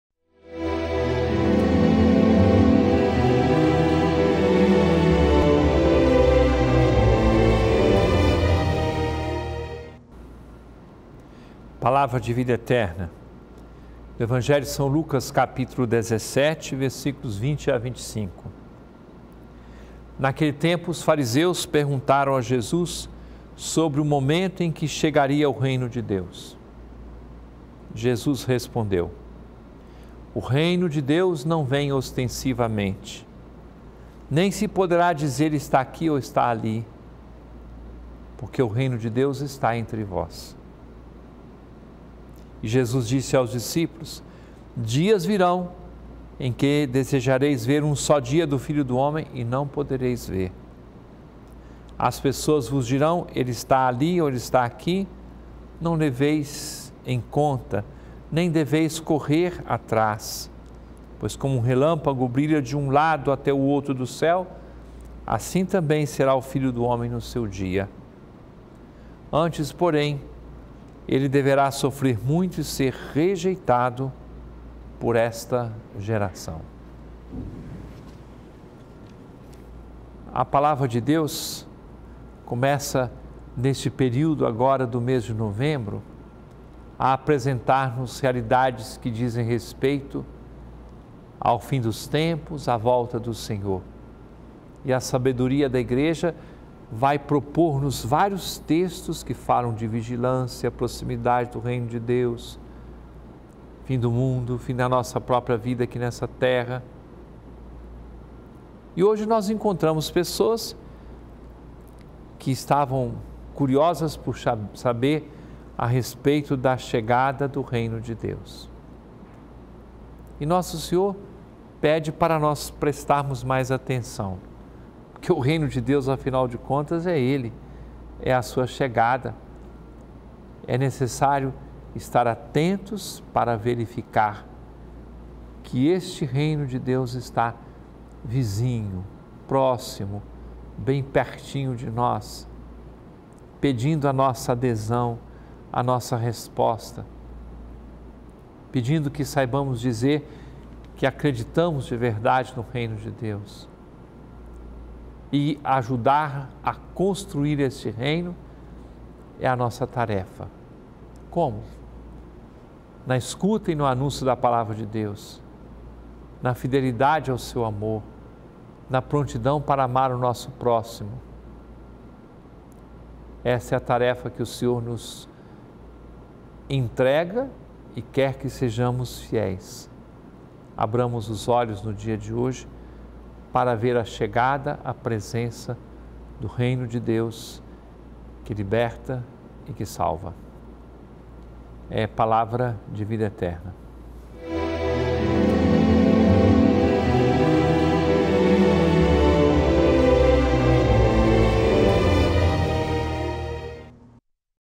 Palavra de Vida Eterna | São Lucas 17,20-25 10/11/2016
Naquele tempo, 20os fariseus perguntaram a Jesus sobre o momento em que chegaria o Reino de Deus. Jesus respondeu: “O Reino de Deus não vem ostensivamente. 21Nem se poderá dizer: ‘Está aqui’ ou ‘Está ali’, porque o Reino de Deus está entre vós”.

22E Jesus disse aos discípulos: “Dias virão em que desejareis ver um só dia do Filho do Homem e não podereis ver. 23As pessoas vos dirão: ‘Ele está ali’ ou ‘Ele está aqui’. Não deveis ir, nem correr atrás. 24Pois, como o relâmpago brilha de um lado até o outro do céu, assim também será o Filho do Homem, no seu dia. 25Antes, porém, ele deverá sofrer muito e ser rejeitado por esta geração”.